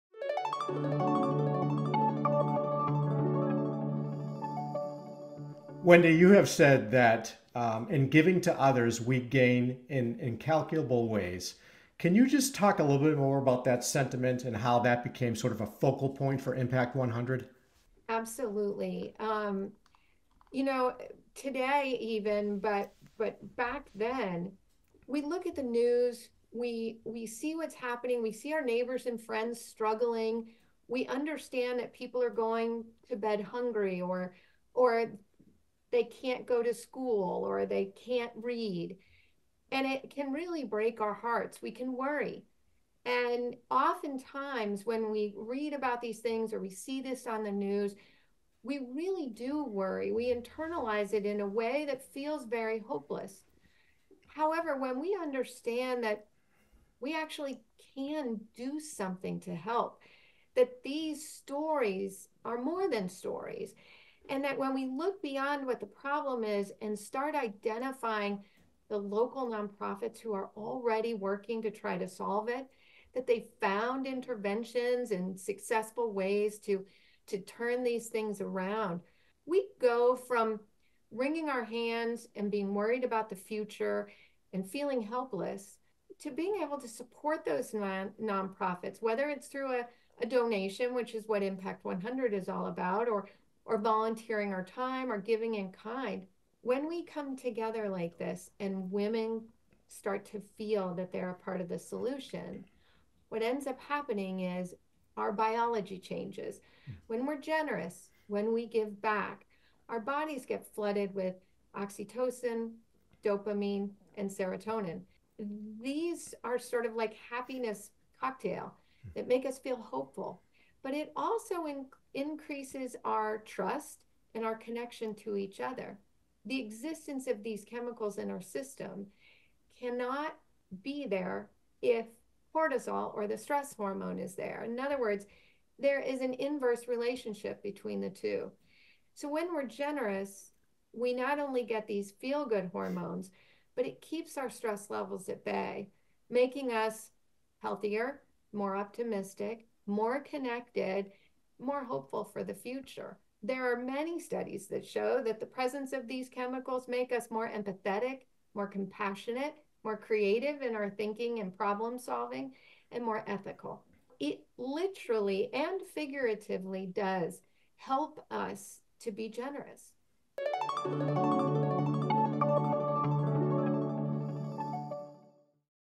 Science Proves Giving Back Actually Makes Humans Happier
Wendy Steele, founder of Impact100, discussed an interesting phenomenon related to giving back: When humans do it, their biology changes in a way that actually makes them happier, more compassionate, and more creative. She shared more on the Beyond Profit podcast.